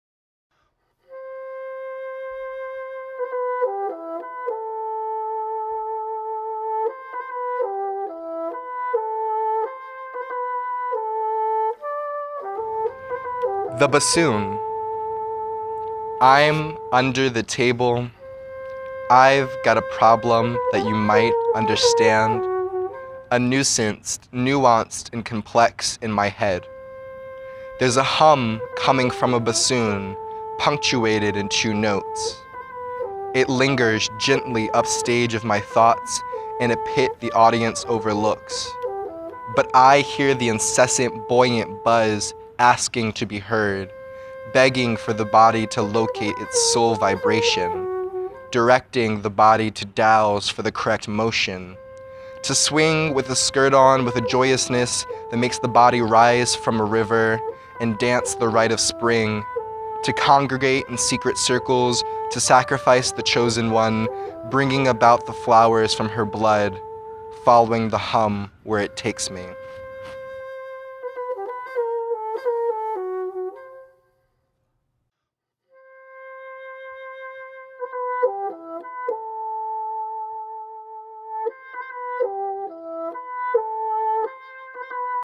The Bassoon
A visual Interpretation of Brandon Blue's "The Bassoon"

Keep an eye at for our first submission date on all social media!

Issu: TickyTackyZine